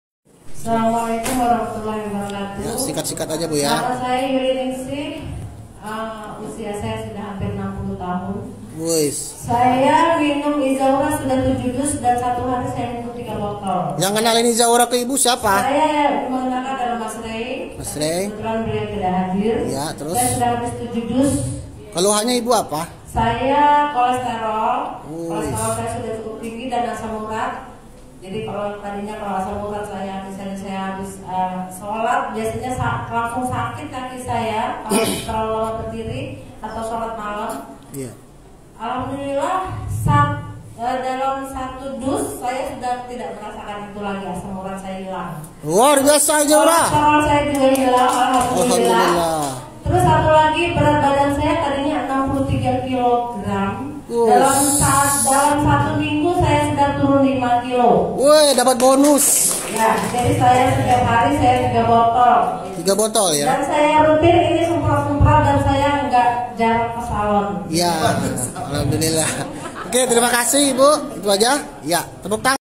Testimoni Bebas Dari Asam Urat dan Kolestrol dgn iZaura
Alhamdulillah

Dengan wasilah izaura di minum rutin 3 botol per hari ibu usia hampir 60 tahun bebas dari keluhan Asam urat dan kolestrol  plus bisa turun berat badan.

Semoga banyak masyarakat yang, terbantu dgn izaura